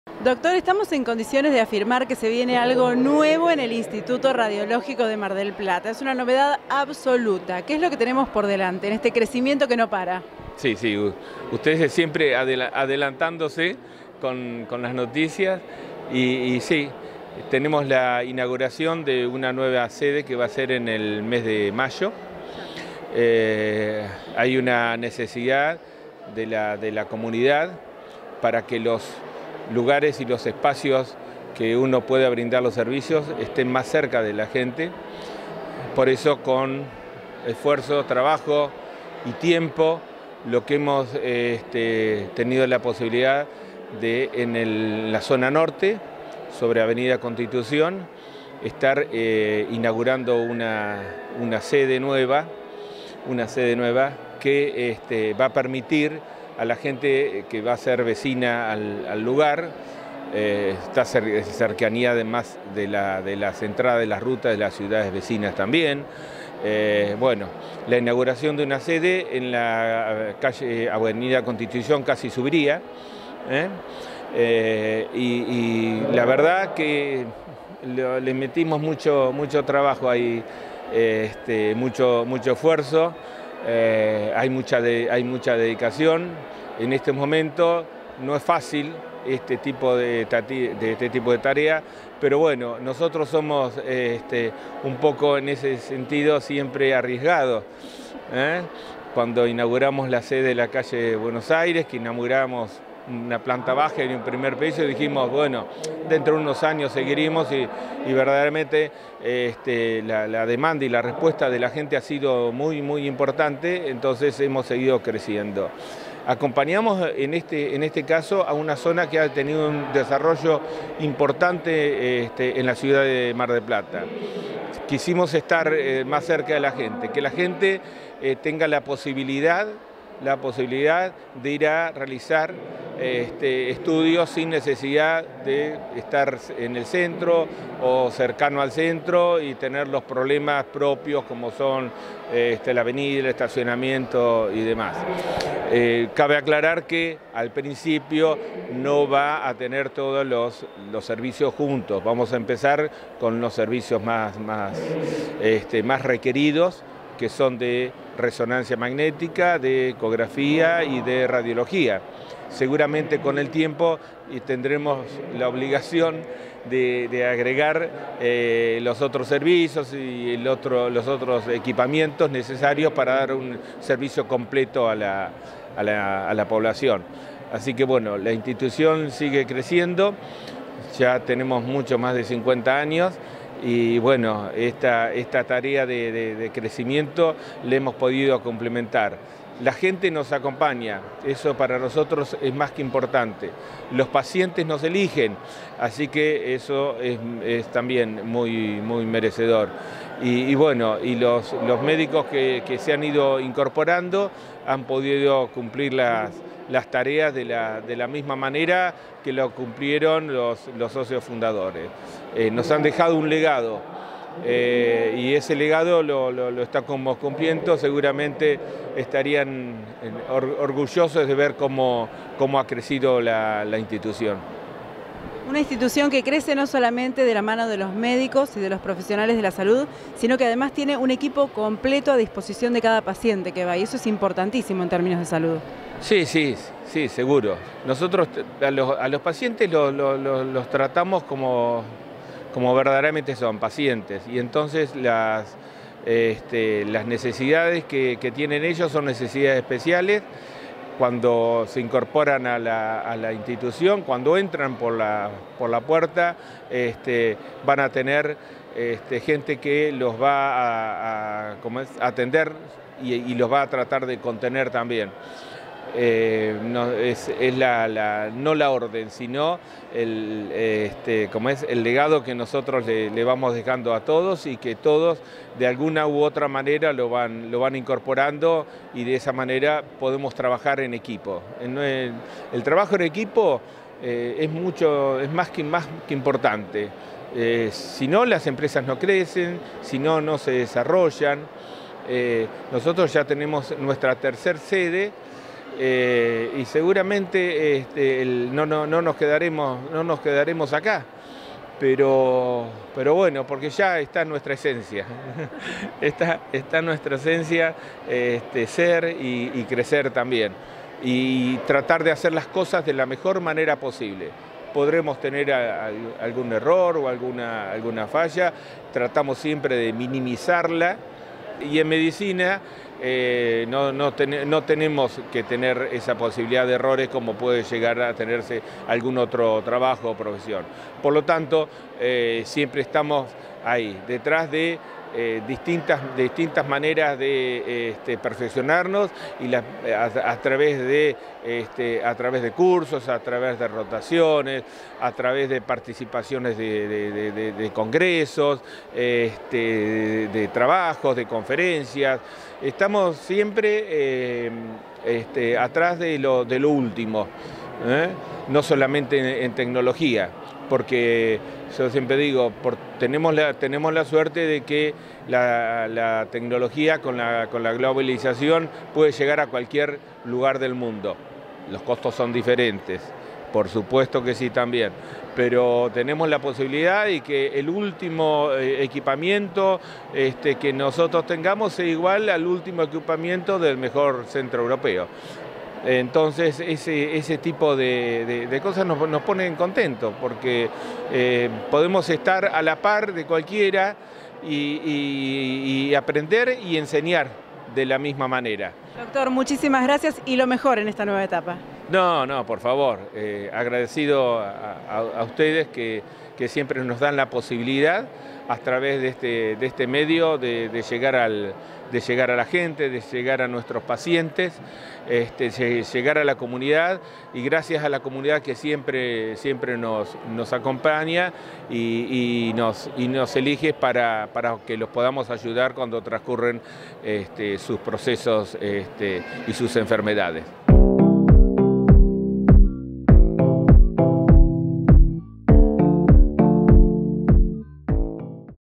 EL INSTITUTO RADIOLÓGICO SIGUE CRECIENDO: ABRIRÁ UNA NUEVA SEDE EN LA ZONA NORTE DE MAR DEL PLATA.
Dr. Juan Carlos Barbería
Médico especialista en diagnóstico por imágenes
Integrante del directorio del Instituto Radiológico